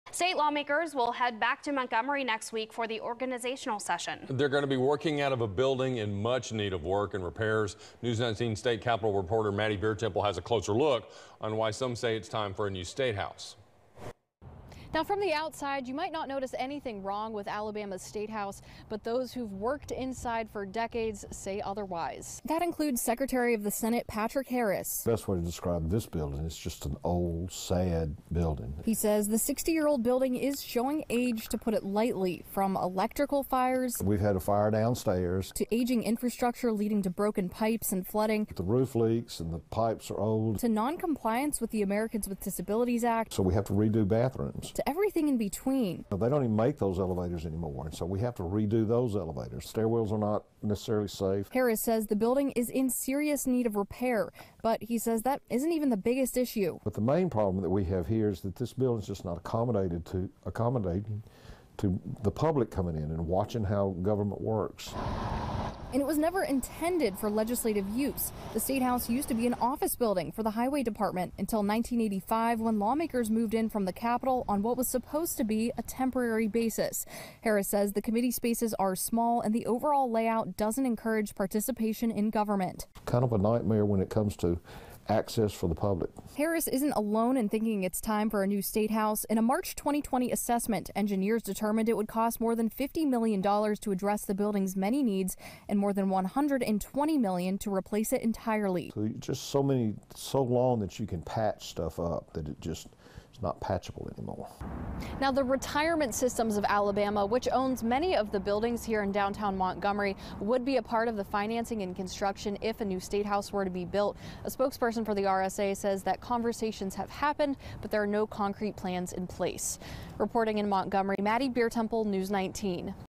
Alabama State House in Need of Repairs
State lawmakers will head back to Montgomery next week for the organizational session, but they'll be working out of a building in much need of repairs